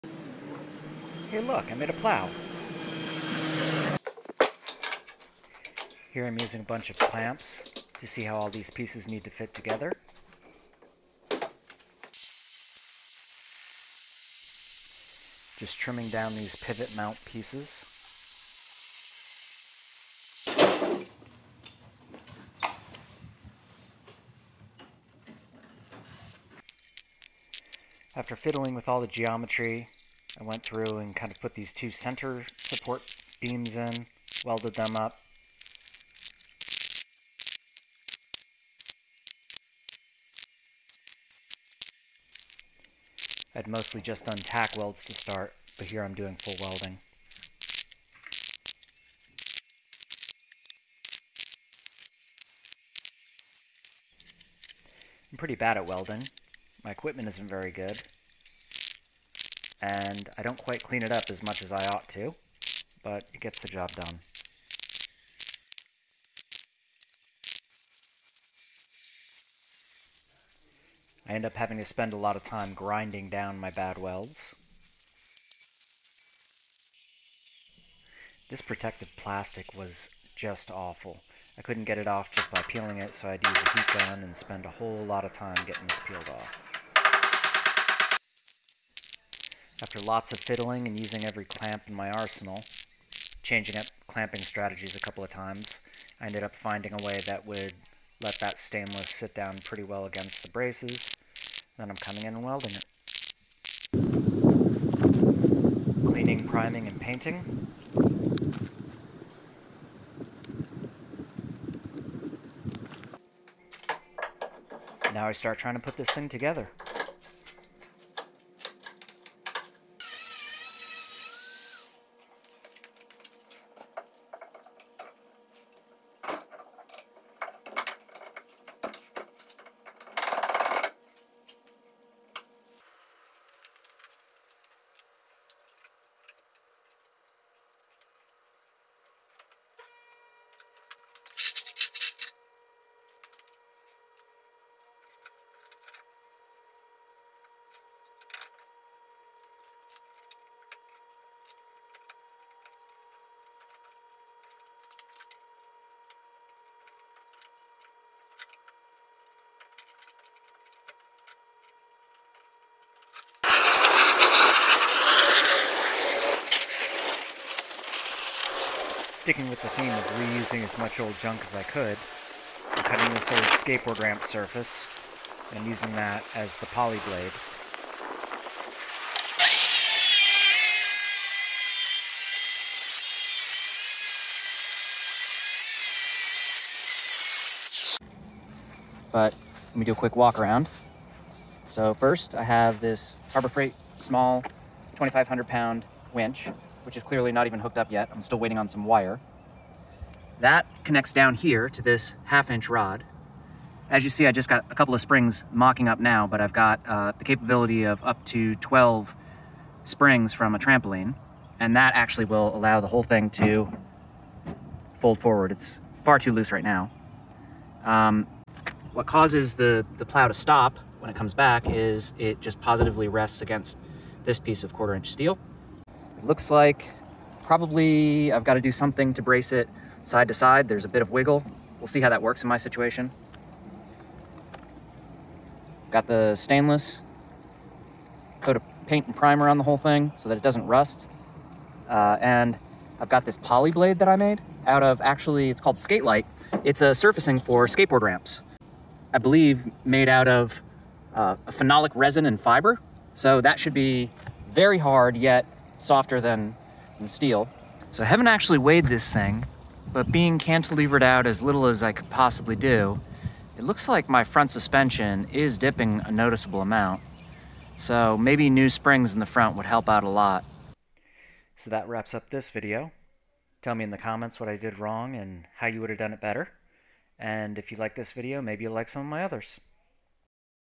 DIY Snow Plow from Trash - Part 2
Cheapskate builds a DIY snow plow for a Suzuki Carry mini kei truck.

Using scraps and very little understanding of how a plow works I build one from scratch. A bit of welding, grinding, and drilling - along with plenty of head scratching. In part 2 I finish the fiddly bits of welding, painting, assembling the plow.